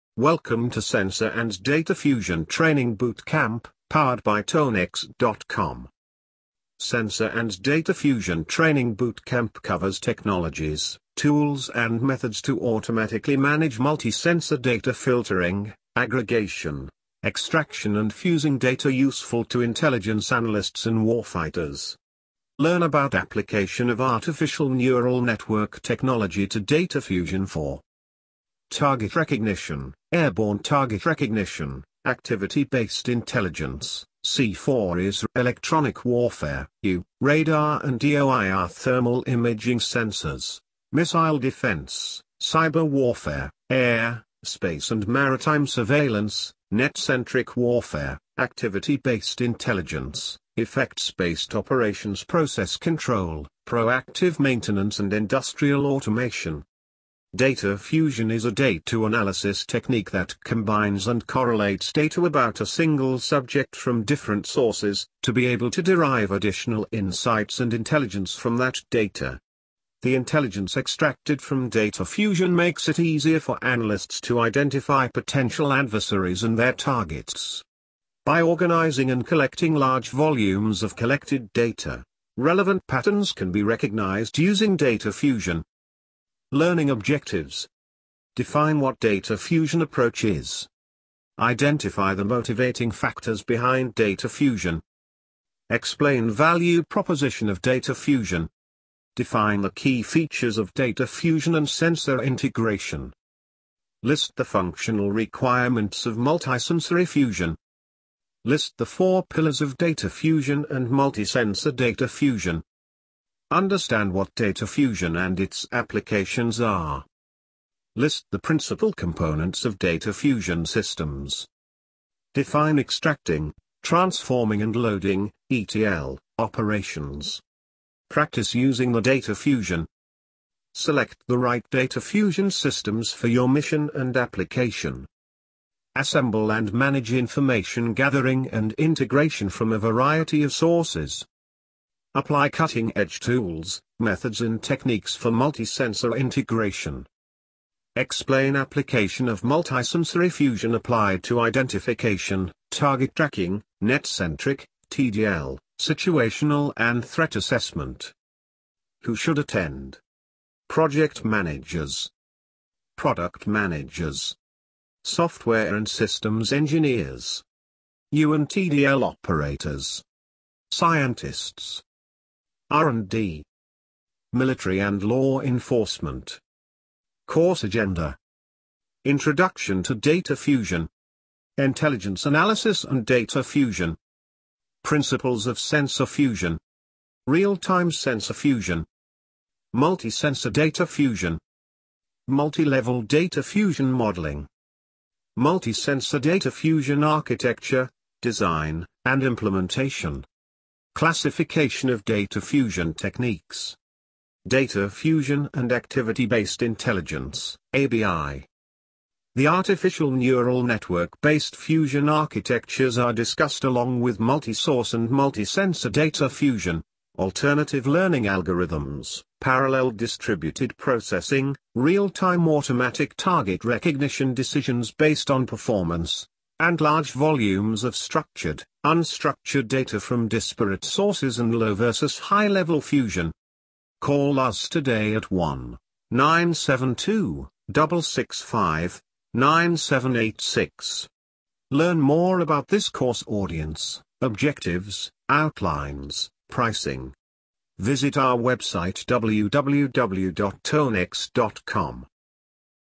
Sensor and Data Fusion Training Bootcamp - Tonex Training Experts
Sensor & Data Fusion training bootcamp covers technologies, tools and methods to automatically manage multi sensor data filtering, aggregation, extraction and fusing data useful to intelligence analysts and war fighters. Learn about application of artificial neural network technology to data fusion .

Learn about application of artificial neural network technology to data fusion for :

target recognition, airborne target recognition, activity-based intelligence, C4ISR, Electronic Warfare (EW), radar and EO-IR thermal imaging sensors, missile defense, cyber warfare, air, space and maritime surveillance, net-centric warfare, Activity-based Intelligence, effects-based operations process control, proactive maintenance and industrial automation.

Audience :

Project managers
Product managers
Software and systems engineers
EW and TDL operators
Scientists
R&D
Military and law enforcement.

Learning Objectives:

Define what data fusion approach is
Identify the motivating factors behind data fusion
Explain value proposition of data fusion
Define the key features of data fusion and sensor integration
List the functional requirements of multisensory fusion
List the four pillars of data fusion and Multi-Sensor Data Fusion
Understand what data fusion and its applications are
List the principal components of data fusion systems
Define Extracting, Transforming and Loading (ETL) operations
Practice using the data fusion
Select the right data fusion systems for your mission and application
Assemble and manage information gathering and integration from a variety of sources
Apply cutting-edge tools, methods and techniques for multi-sensor integration
Explain application of multisensory fusion applied to identification, target tracking, net-centric, TDL, situational and threat assessment

Learn more about this course audience, objectives, outlines, pricing.